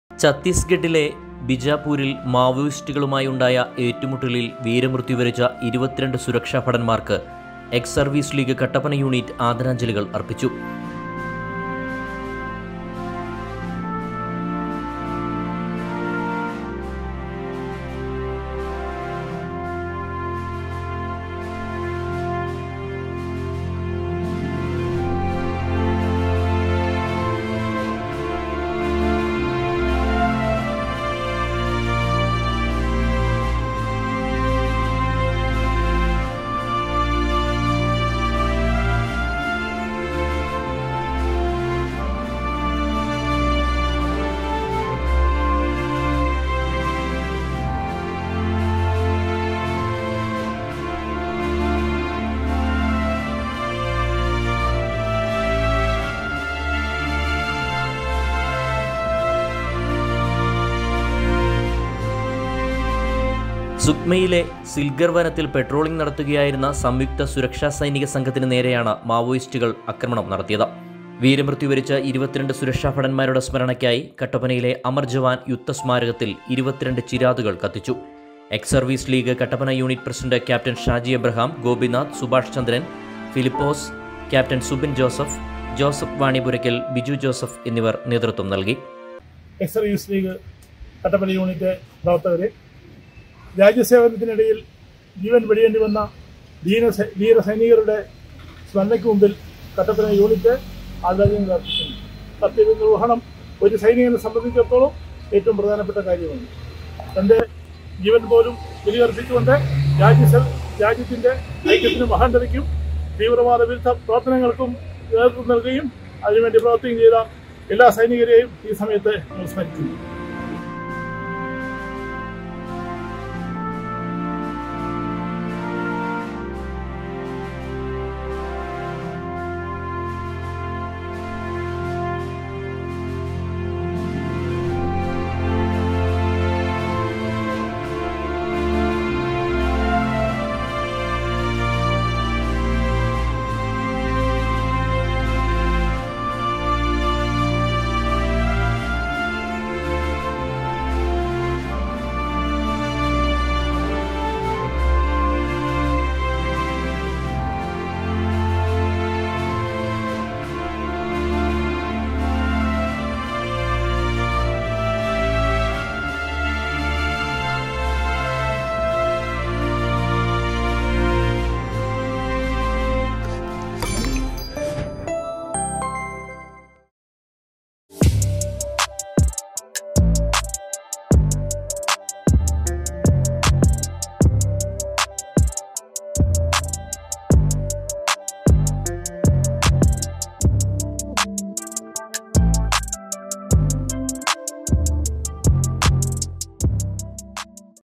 വീരമൃത്യു വരിച്ച 22 സുരക്ഷാഭടന്മാരുടെ സ്മരണക്കായി 22 ചിരാതുകൾ കത്തിച്ചു
ഛത്തീസ്ഗഡിലെ ബിജാപ്പൂരിൽ മാവോയിസ്റ്റുകളുമായി ഉണ്ടായ ഏറ്റുമുട്ടലില്‍ വീരമൃത്യു വരിച്ച 22 സുരക്ഷാഭടന്മാര്‍ക്ക് എക്‌സ് സർവീസ് ലീഗ് കട്ടപ്പന യൂണിറ്റ് ആദരാഞ്ജലികൾ അർപ്പിച്ചു. സുക്മയിലെ സില്‍ഗര്‍ വനത്തില്‍ പട്രോളിങ് നടത്തുയായിരുന്ന സംയുക്ത സുരക്ഷ സൈനിക സംഘത്തിന് നേരെയാണ്   മാവോയിസ്റ്റുകള്‍ ആക്രമണം നടത്തിയത്.